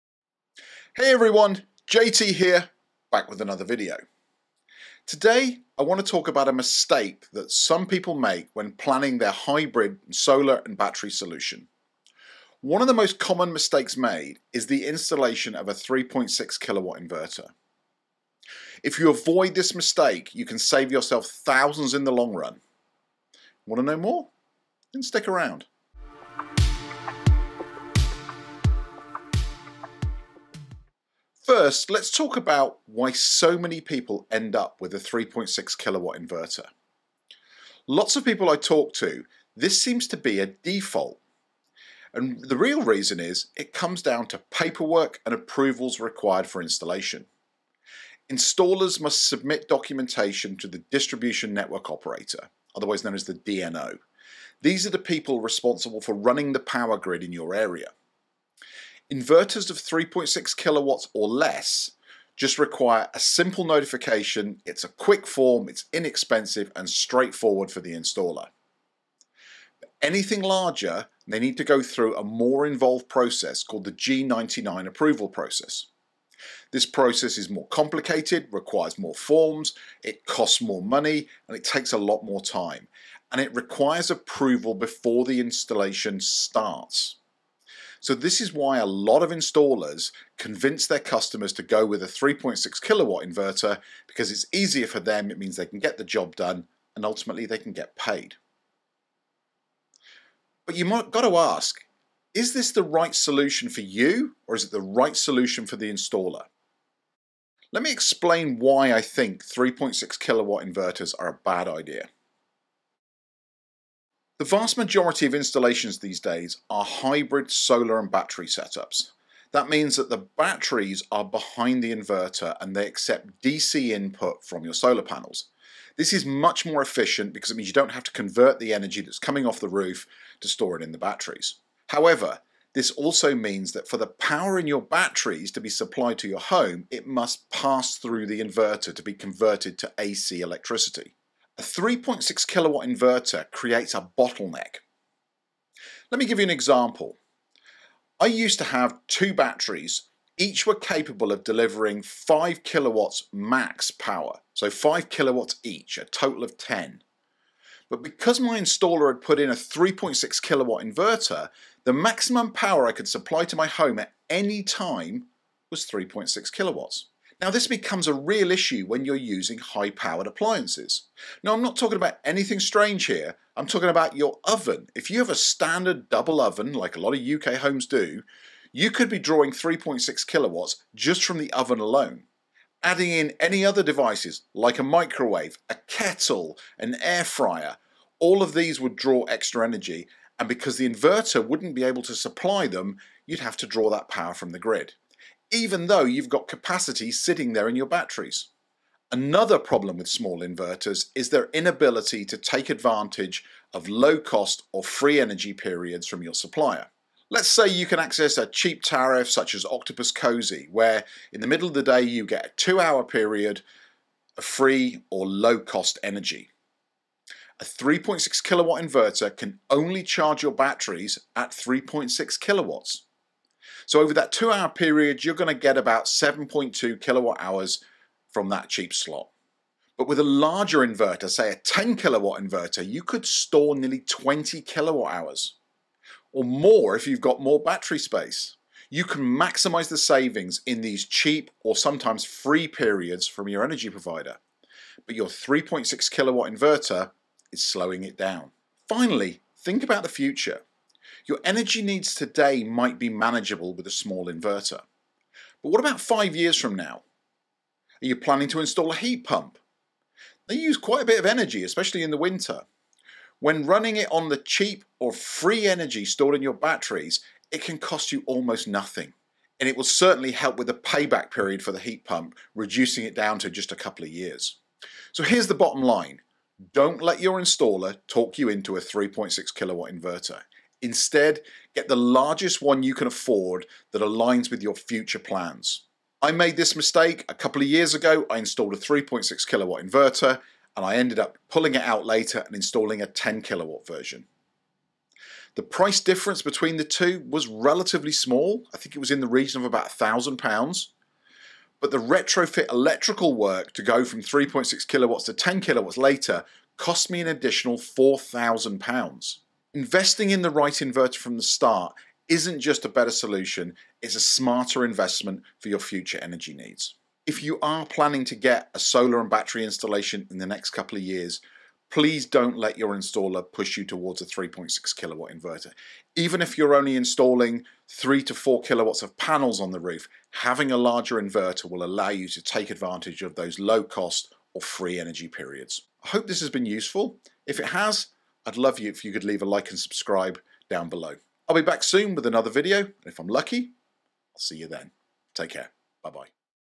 Want Energy Efficiency? Watch This Hybrid Inverter Breakdown Now!
Most solar installation companies in the UK install 3.6kw hybrid inverters because it is easy and convenient for the installer, but it is terrible for the customer. You will regret it if you install a 3.6kw inverter sooner rather than later.

1️⃣Shelly EM Pro 50 - Energy Monitor
2️⃣Aqara Temperature and Humidity Sensor Kit - 3pcs
3️⃣Aqara Temperature and Humidity Sensor, Single Unit

00:00 Introduction
00:22 save yourself thousands of £££
00:37 Why does everyone have a 3.6kw inverter
01:12 G99 Approval
01:42 Is this right for the customer?
01:52 Why 3.6kw inverters are a terrible idea
02:27 My own mistake
02:56 Why 3.6kw isn't enough
03:20 It is Useless for chargeing batteries quickly
04:10 Your future energy use
04:35 The bottom line